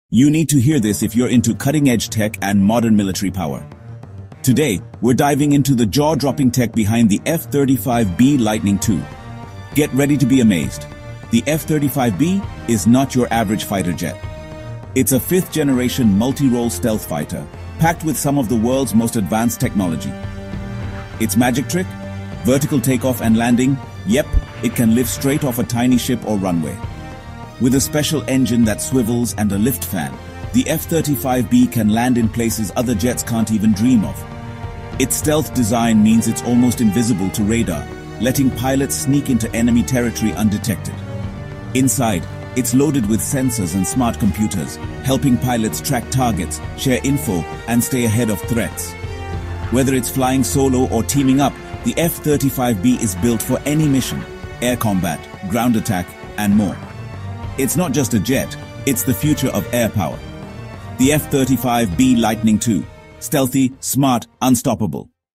F-35B Lightning II in Action – The World’s Most Advanced Stealth Fighter Explained!
Experience the power of the F-35B Lightning II – the world’s first supersonic stealth fighter with short takeoff and vertical landing (STOVL) capability.
In this video, we break down everything you need to know about this fifth-generation multi-role jet used by the U.S. Marine Corps, Royal Navy, and other allied forces.

🔍 What You’ll Learn in This Video:

Key features and stealth technology

Vertical takeoff and landing demo

Real combat capabilities

Comparison with other fighter jets like F-22, Su-57, and Rafale

Cost, speed, and weapon systems

💥 Why is the F-35B a game-changer in modern air warfare? Find out now!